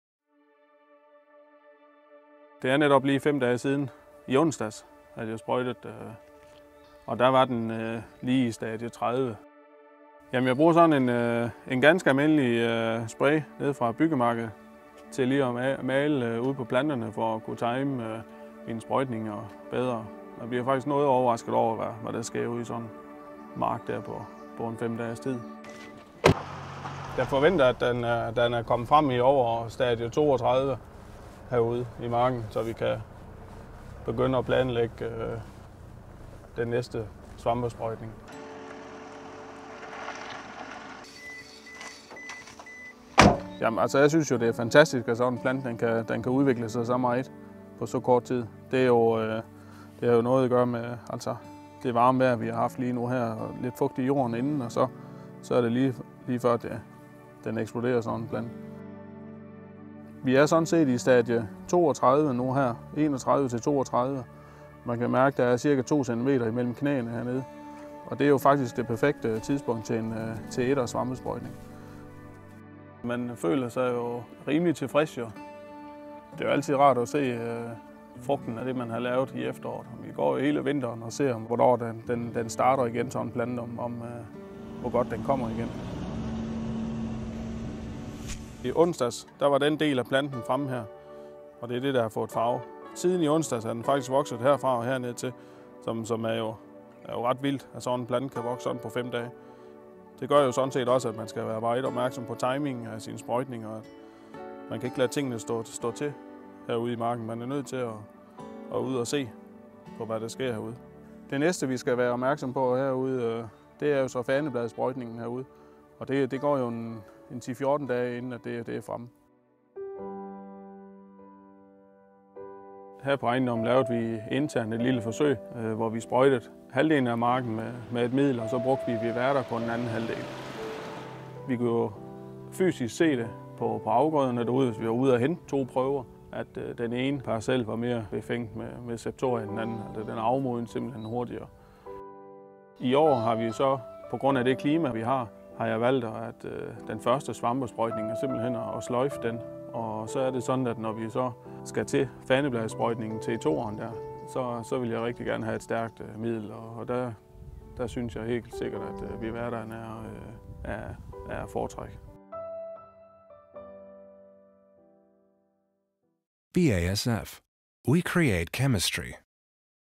Driftsleder: Mal planten for optimal timing
Driftsleder Kasper Henricksen på en større planteavlsbedrift på Samsø, er afhængig af effektiv svampebekæmpelse for at kunne leve op til kravene om højt udbytte.